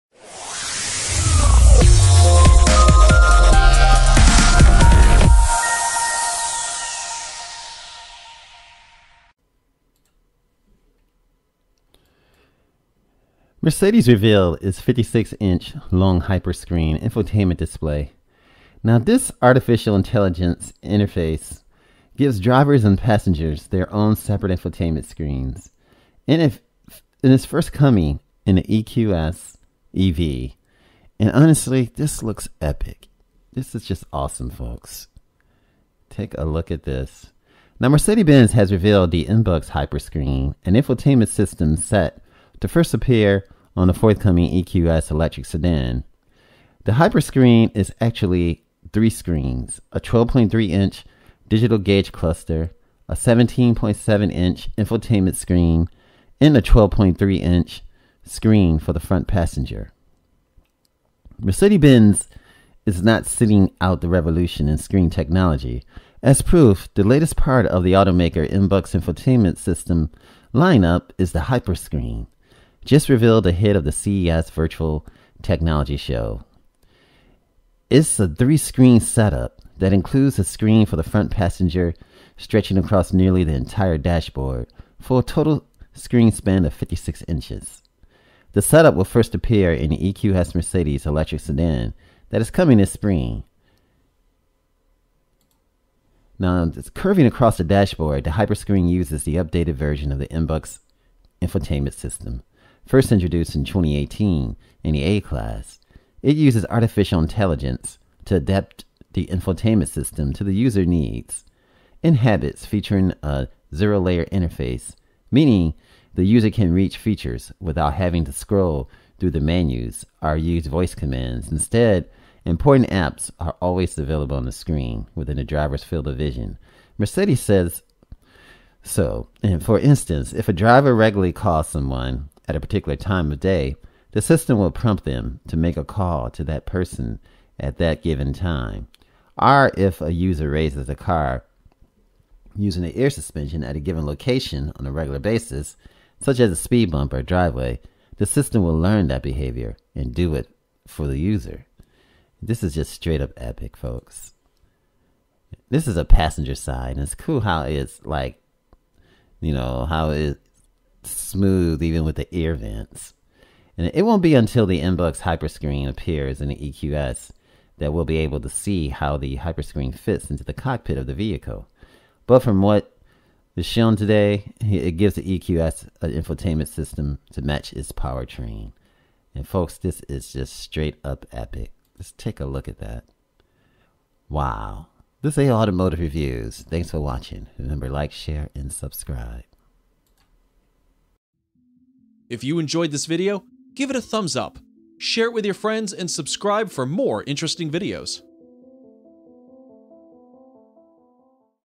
Mercedes Reveals Its 56-Inch-Long Hyperscreen Infotainment Display
Mercedes-Benz is not sitting out the revolution in screen technology. As proof, the latest part of the automaker's MBUX infotainment lineup is Hyperscreen. Just revealed ahead of the CES virtual technology show next week, it's a three-screen setup that includes a screen for the front passenger, stretching across nearly the entire dashboard for a total screen span of 56 inches.The setup will first appear in the EQS, Mercedes's electric sedan that is coming this spring.